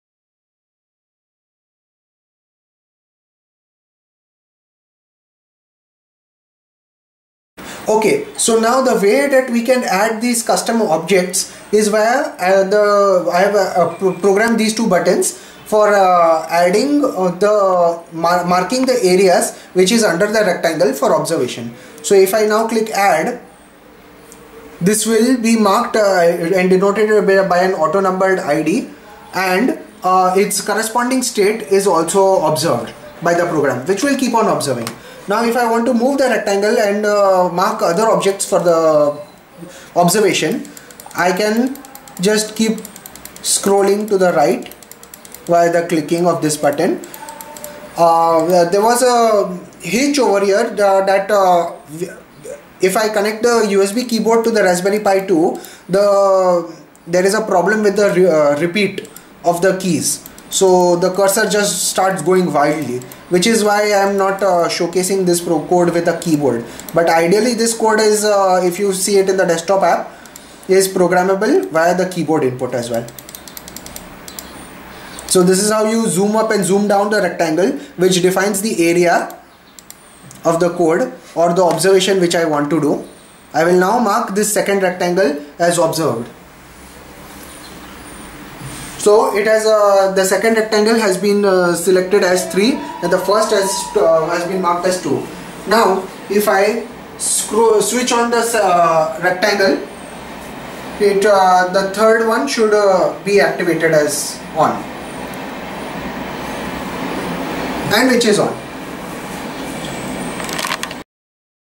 Custom Object Tracking using Microsoft CNTK Raspbery pi 2
Tutorial highlights How i Mark area of observation for the program to identify and track switches as off/on

Article Link
Part 1